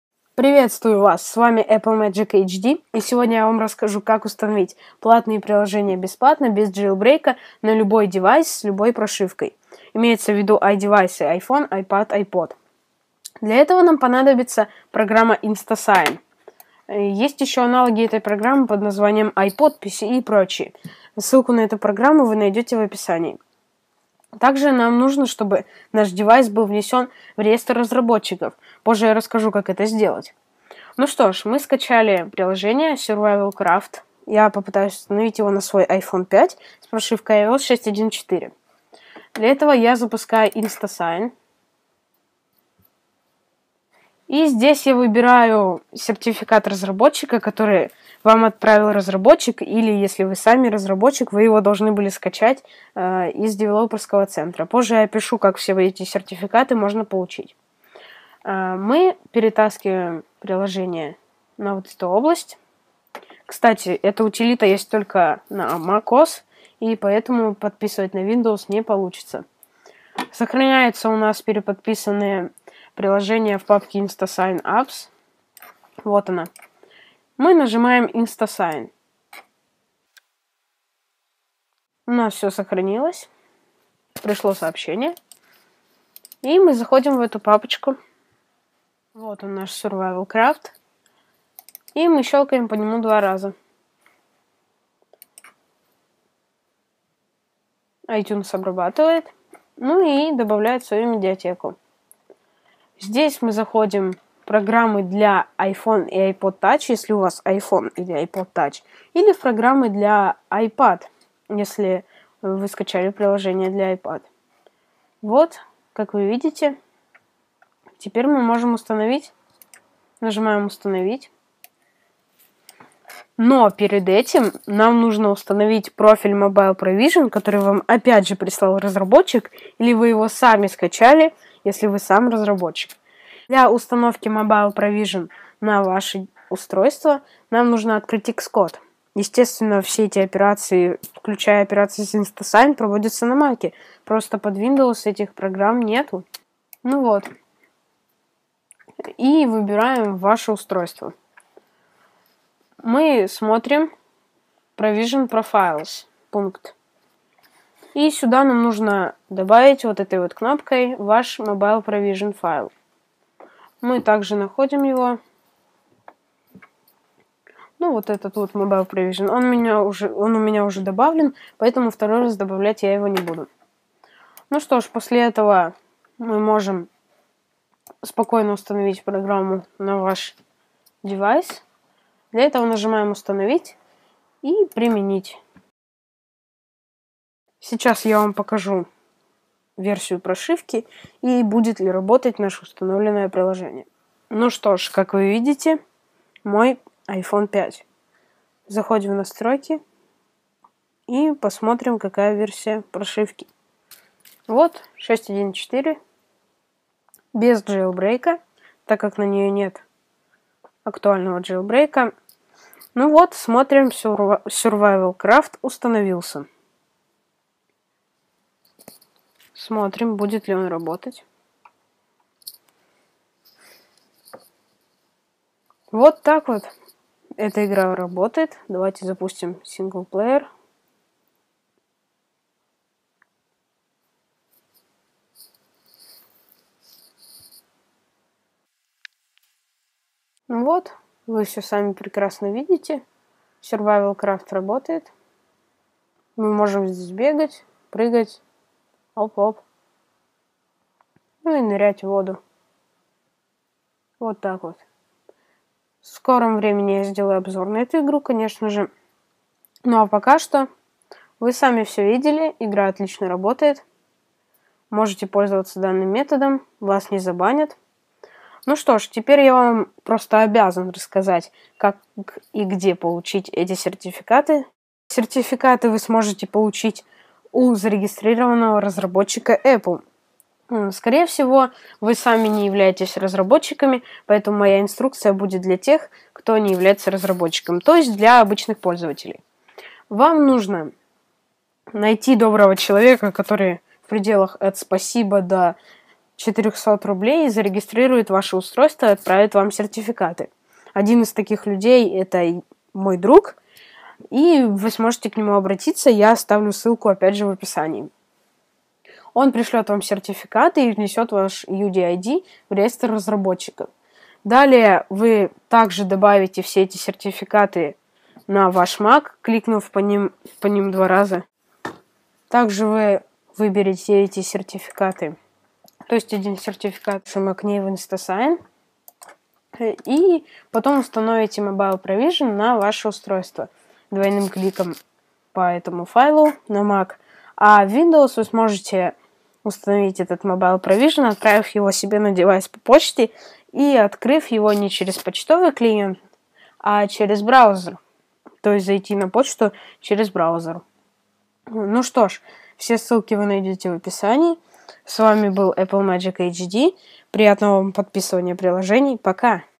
Установка платных приложений бесплатно без JB
В этом видео я рассказываю про способ установки платных приложений бесплатно без Jailbreak. Вам понадобится приложение InstaSign так же вам понадобится зарегистрировать ваш девайс у разработчика и попросить его выслать вам сертификаты. Разработчик Приложения можно скачивать отсюда